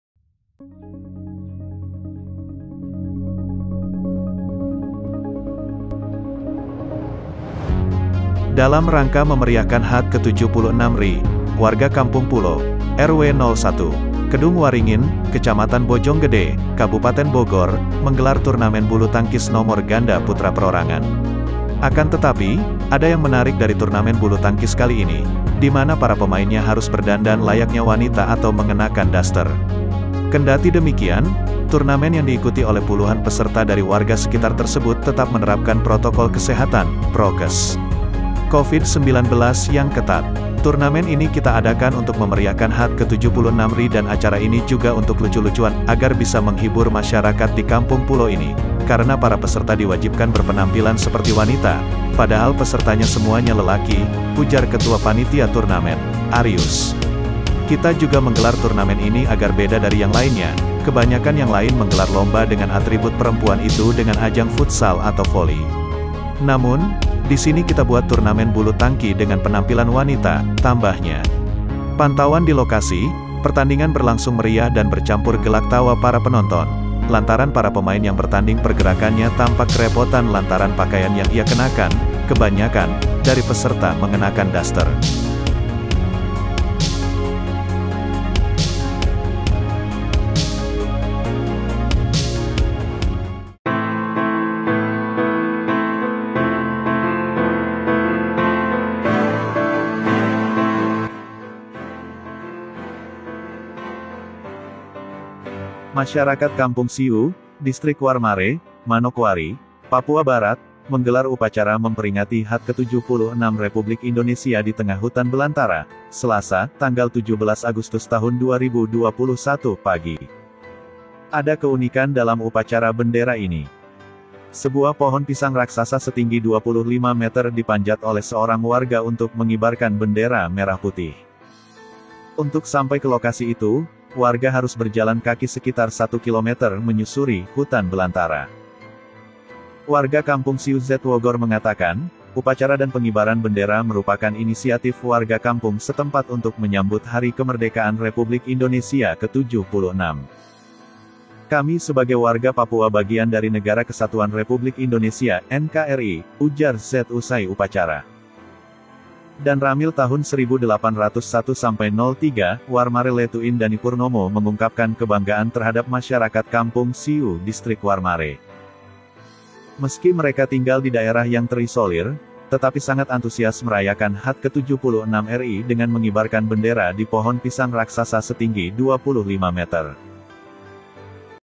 Viral Turnamen Bulutangkis Putra Mengenakan Daster Memeriahkan HUT Ke 76 RI
LAMPUNG.TRIBUNNEWS.COM - Dalam rangka memeriahkan HUT Ke-76 RI, warga Kampung Pulo, RW 01,  Kedung Waringin, Kecamatan Bojonggede, Kabupaten Bogor, menggelar turnamen bulutangkis nomor ganda putra perorangan.
Akan tetapi, ada yang menarik dari turnamen bulutangkis kali ini, di mana para pemainnya harus berdandan layaknya wanita atau mengenakan daster.
Kendati demikian, turnamen yang diikuti oleh puluhan peserta dari warga sekitar tersebut tetap menerapkan protokol kesehatan (prokes) Covid-19 yang ketat.
"Turnamen ini kita adakan untuk memeriahkan HUT Ke-76 RI dan acara ini juga untuk lucu-lucuan, agar bisa menghibur masyarakat di Kampung Pulo ini, karena para peserta diwajibkan berpenampilan seperti wanita, padahal pesertanya semuanya lelaki," ujar Ketua Panitia Turnamen, Arius.
"Kita juga menggelar turnamen ini agar beda dari yang lainnya, kebanyakan yang lain menggelar lomba dengan atribut perempuan itu dengan ajang futsal atau voli. Namun, disini kita buat turnamen bulu tangki dengan penampilan wanita," tambahnya.
Pantauan di lokasi, pertandingan berlangsung meriah dan bercampur gelak tawa para penonton, lantaran para pemain yang bertanding pergerakannya tampak kerepotan lantaran pakaian yang ia kenakan, kebanyakan, dari peserta mengenakan daster.
Artikel ini telah tayang di TribunnewsBogor.com